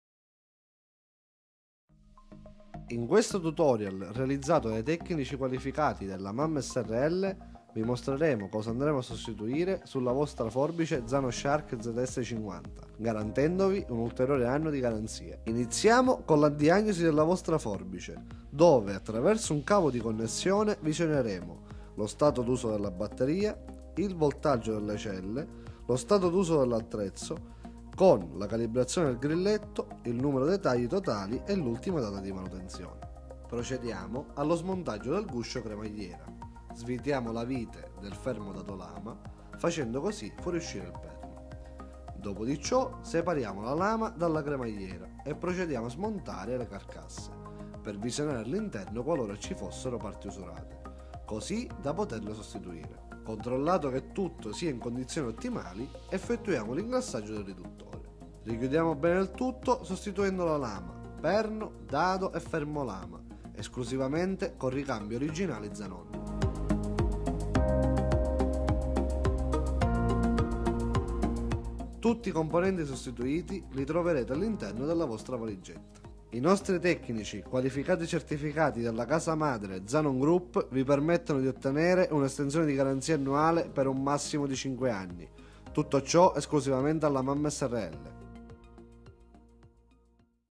REVISIONE FORBICE ZANON SHARK 50 - ESTENSIONE GARANZIA
Video dimostrativo sulla revisione della Forbice Zanon Shark 50.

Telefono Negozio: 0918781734
Responsabile vendite: Michele Provenzano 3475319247